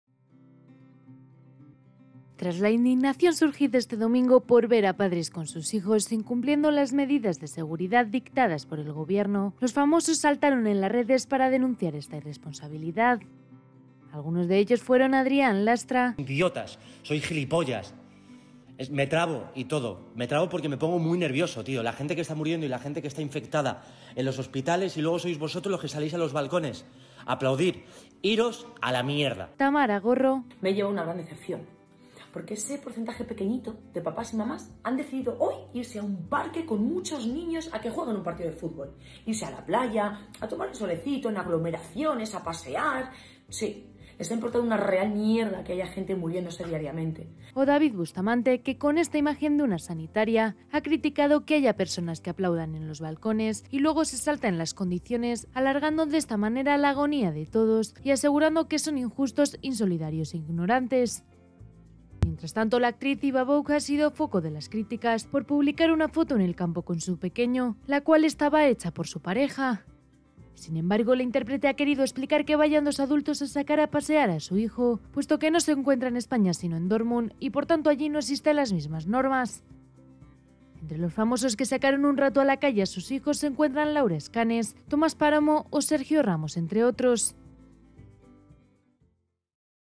Famosos saltan en las redes tras ver "irresponsabilidad" de padres
Tras la indignación surgida este domingo por ver a padres con sus hijos incumpliendo las medidas de seguridad dictadas por el Gobierno, los famosos saltaron a las redes sociales para denunciar esta "irresponsabilidad". Algunos de ellos fueron Adrián Lastra, Tamara Gorro o David Bustamante.